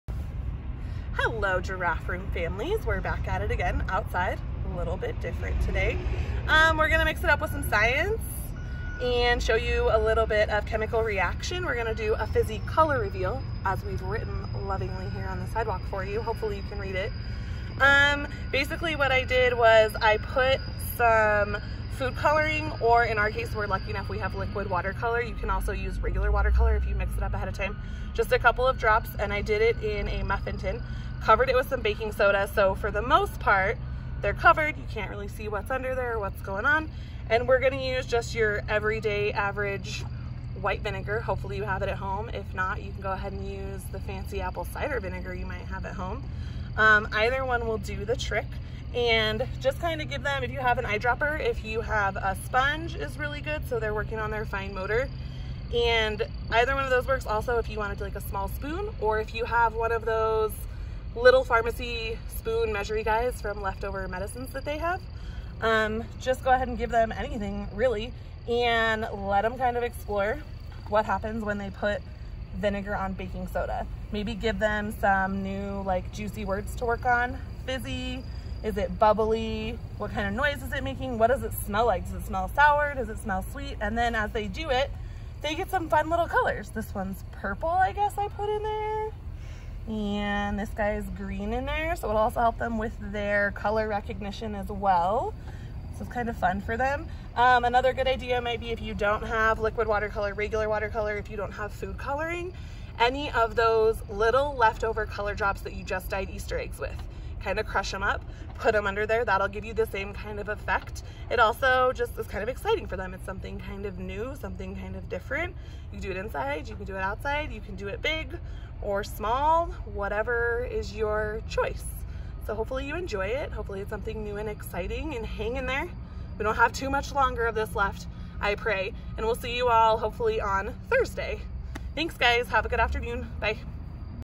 Fizzy Color Reveal with the Giraffe Teacher
Join the Giraffe room teacher as she shows you a chemical reaction experiment with food coloring, baking soda, and vinegar to expose the hidden color.